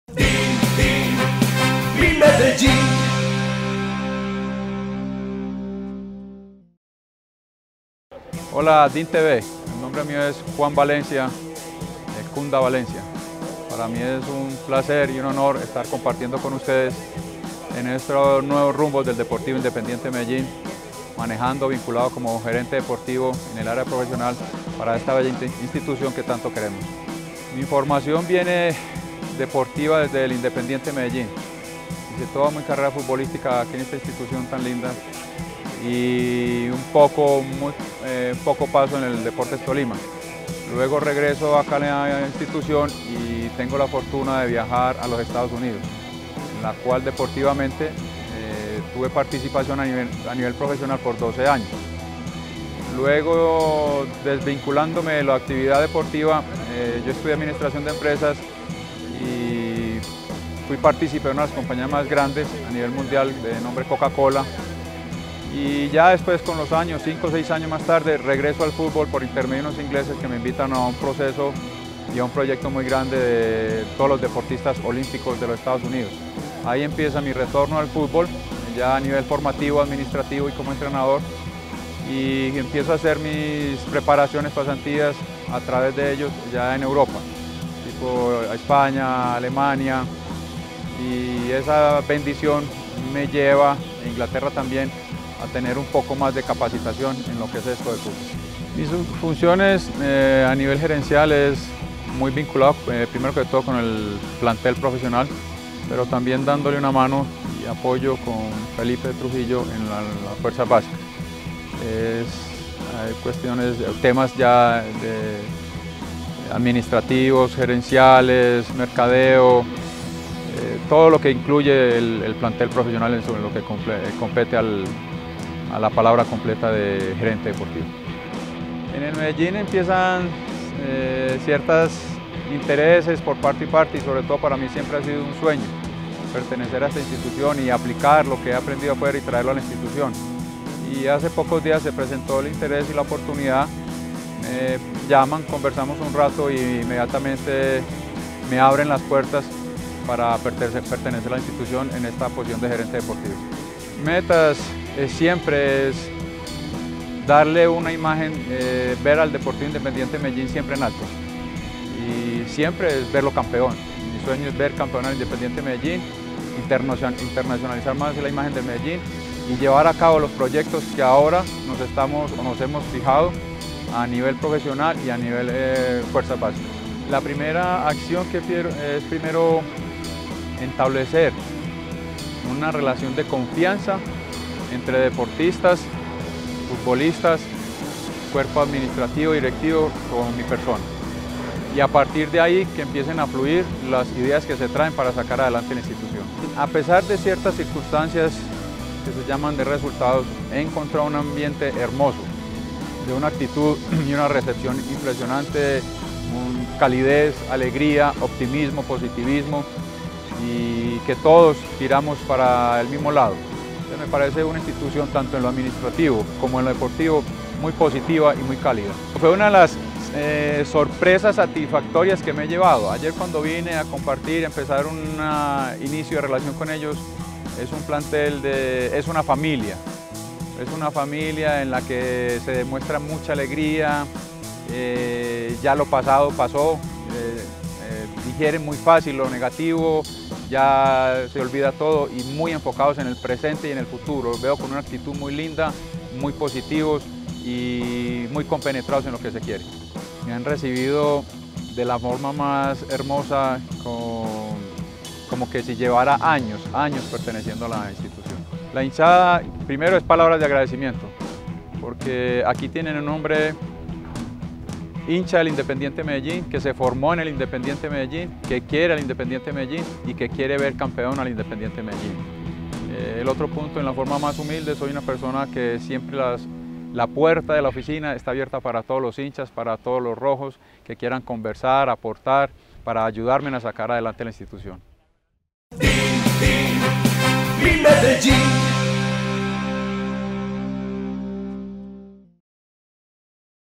DIMTV | Juan Bernardo "Cunda" Valencia - Gerente Deportivo de la División Profesional
Juan Bernardo se formó como futbolista en las fuerzas básicas del DIM y su carrera profesional la adelantó exitosamente en los Estados Unidos e Inglaterra.
Habló con las cámaras de DIMTV y esto fue lo que nos contó.